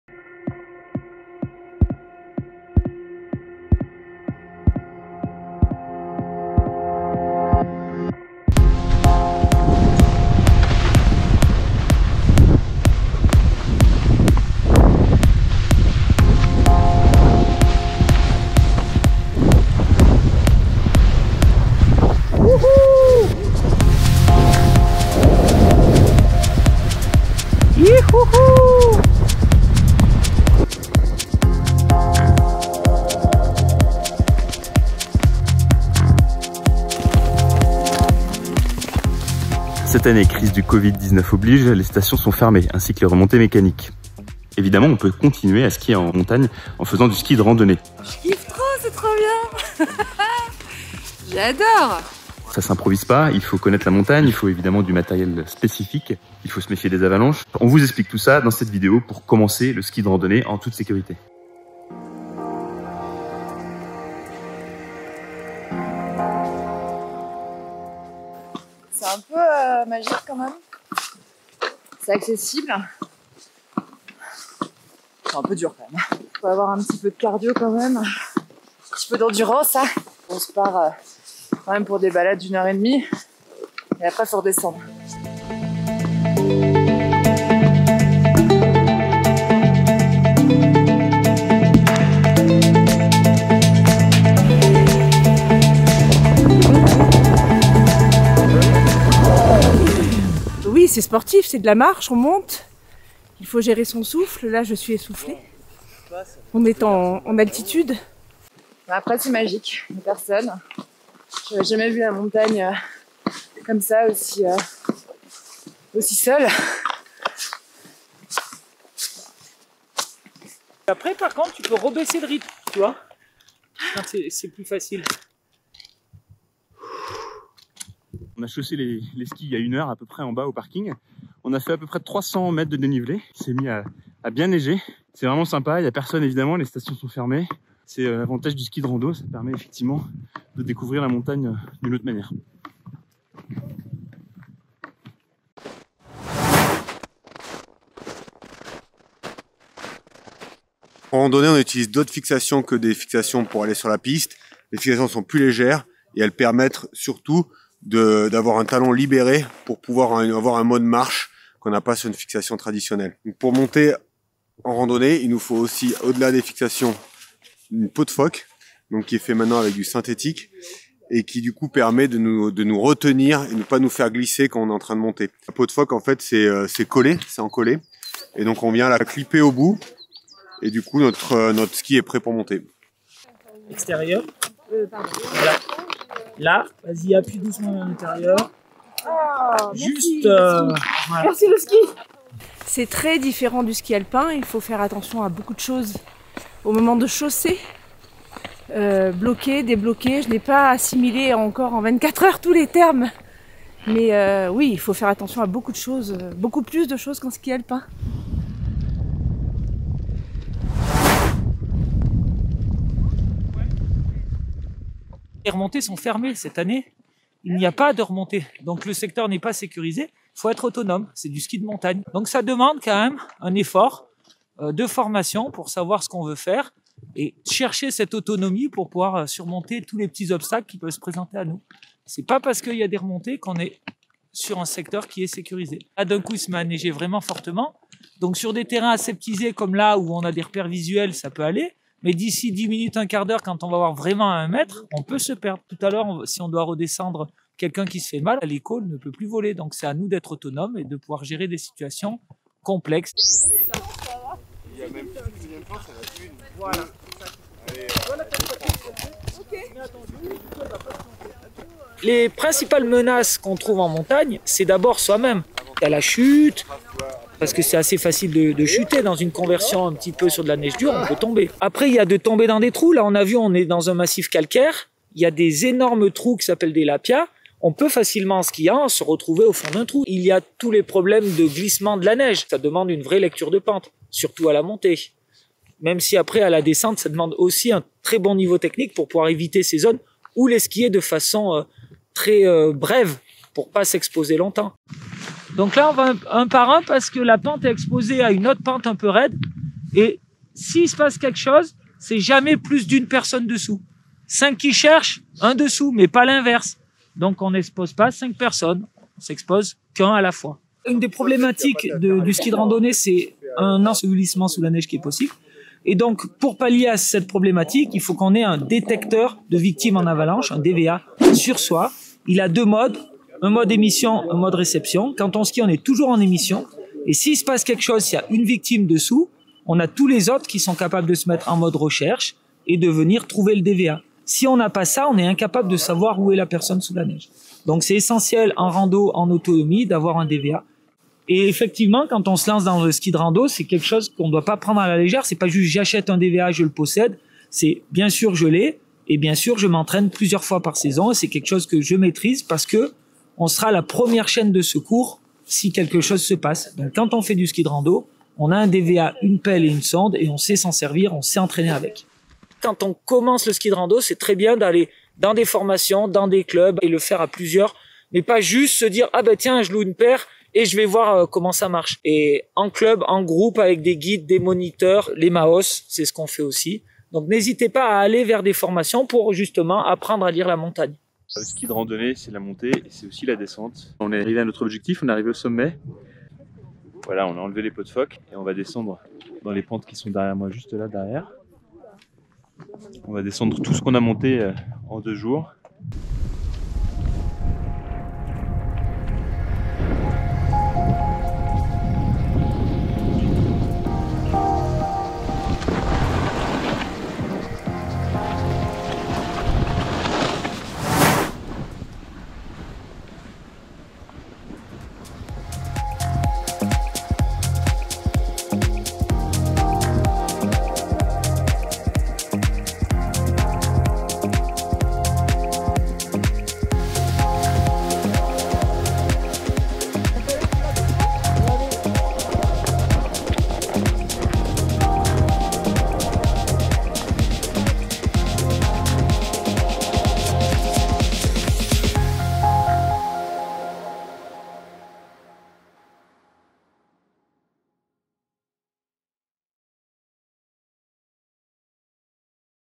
Fermeture des stations de ski : comment passer au ski de randonnée sans risque
La fermeture des stations de sports d’hiver a entraîné le boom du ski de randonnée. Les pistes et les remontées mécaniques sont fermées, mais pour se lancer dans cette pratique sportive qui permet d’évoluer en liberté en montagne, il faut d’abord se former afin d’éviter les pièges de la montagne. Nous avons suivi une formation MAOS organisée par Salomon avec le guide MicheLa. A notre programme, repérer les pentes avalancheuses, les trous cachés dans le manteau neigeux ou même organiser efficacement les secours lorsque une avalanche survient. Mais avant de s’aventurer en ski de randonnée, il faut aussi tout simplement savoir. Chausser ses skis. Régler ses fixations ou installer ses peaux de phoques efficacement ne s’improvisent pas, non plus.